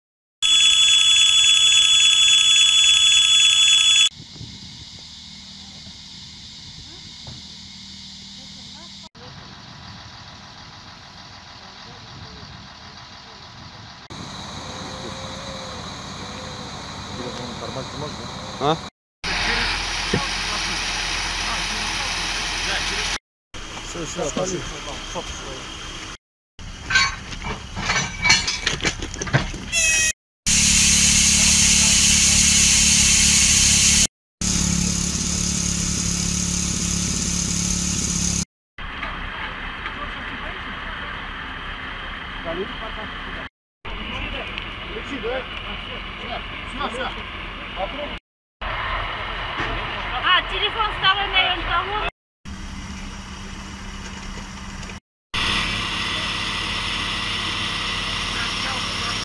В Башкирии легковой автомобиль столкнулся с электричкой
5 марта, в Архангельском районе Башкирии произошла страшная авария. В ДТП скончались два человека. Об этом сообщает пресс-служба ГИБДД по республике